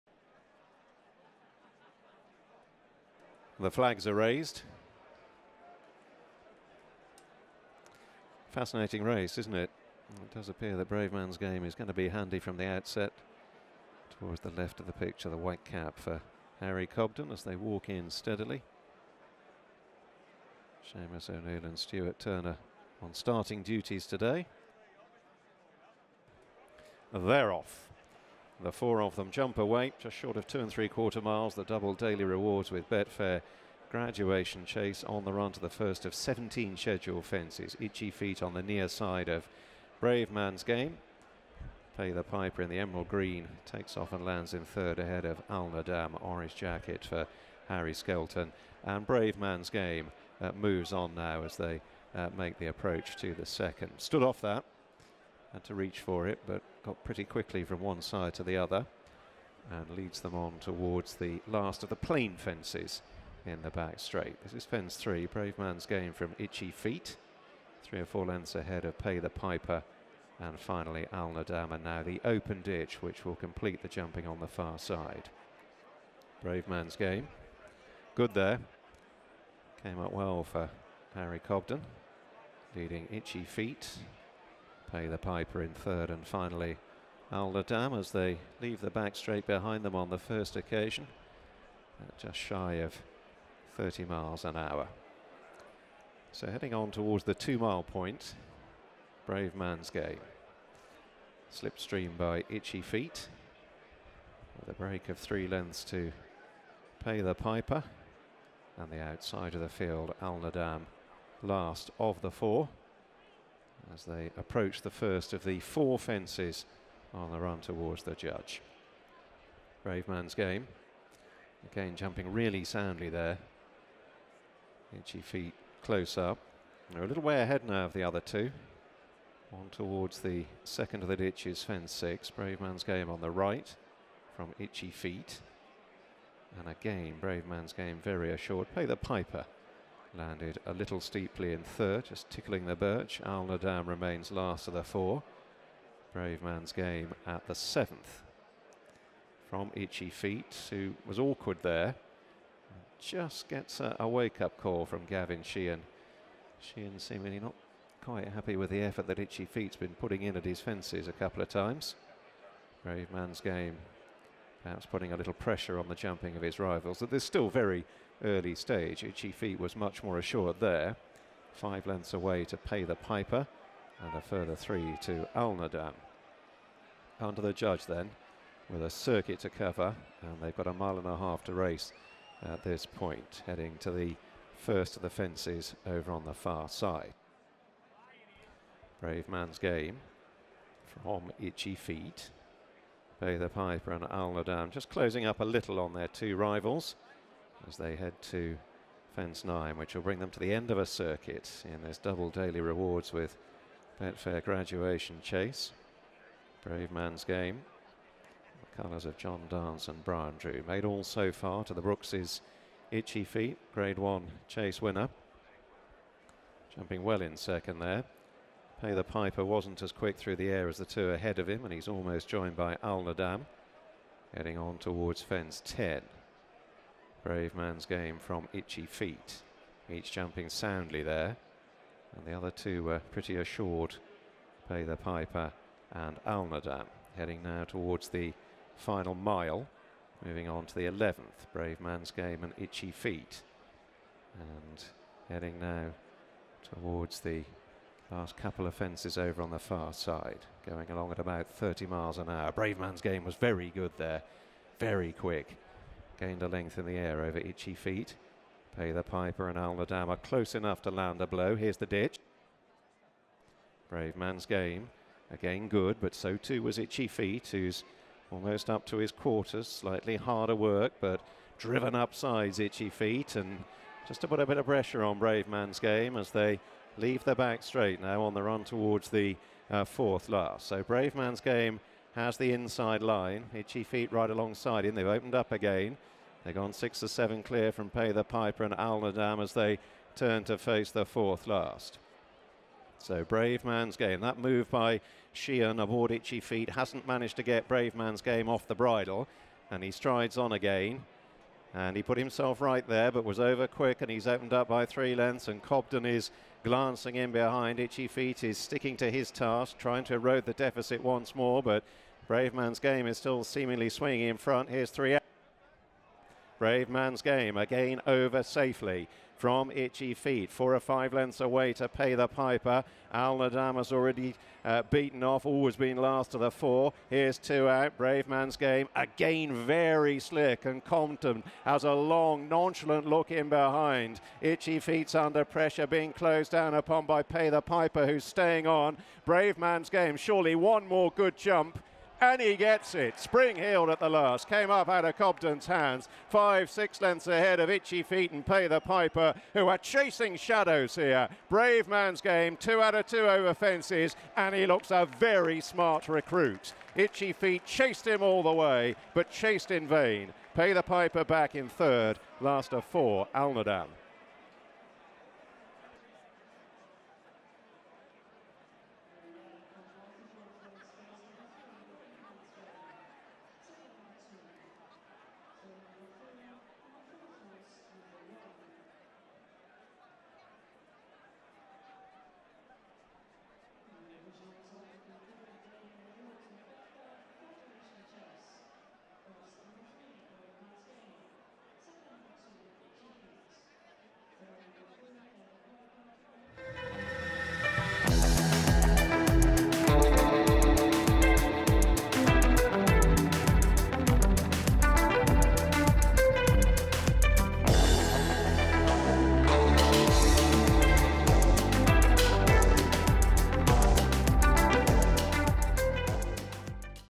BRAVEMANSGAME jumps brilliantly on his way to victory in the Betfair Graduation Chase at Haydock
Paul Nicholls said that he already has a tilt at next year’s King George in mind for Bravemansgame after another faultless display in the Double Daily Rewards With Betfair Graduation Chase at Haydock.

Winner of the Challow Hurdle last winter before placing at Cheltenham and Aintree in the spring, the six-year-old looks like scaling even greater heights over fences judged on his first efforts.

Having readily accounted for the high-class Fusil Raffles on his chasing debut at Newton Abbot last month, the six-year-old faced a similarly stern test on Merseyside.

But Bravemansgame jumped immaculately in front in the hands of Harry Cobden and passed the post with just under three lengths in hand over his 155-rated rival Itchy Feet without being fully extended.